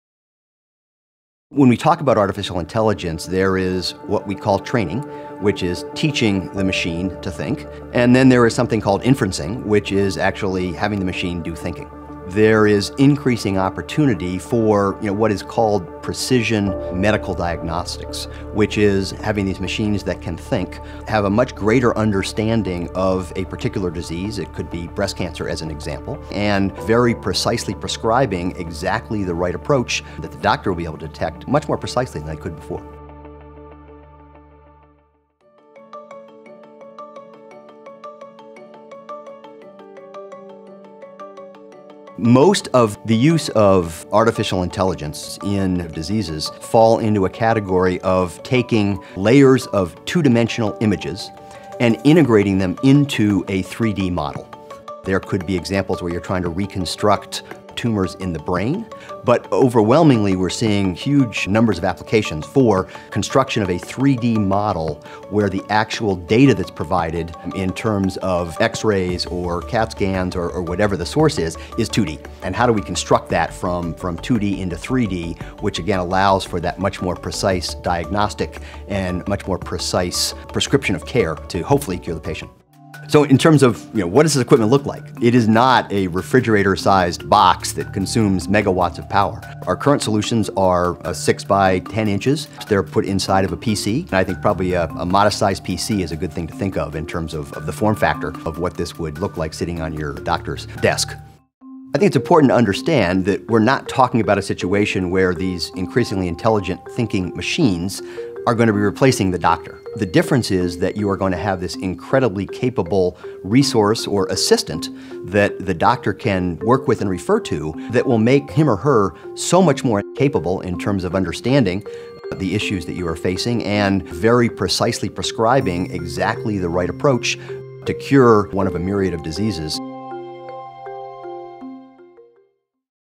How AI helps doctors diagnose diseases more accurately
Artificial intelligence is used to create 3D imaging models that allow for a faster and more accurate diagnosis and treatment of medical conditions. Here's how it works.

MarketWatch provides the latest stock market, financial and business news. Get stock market quotes, personal finance advice, company news and more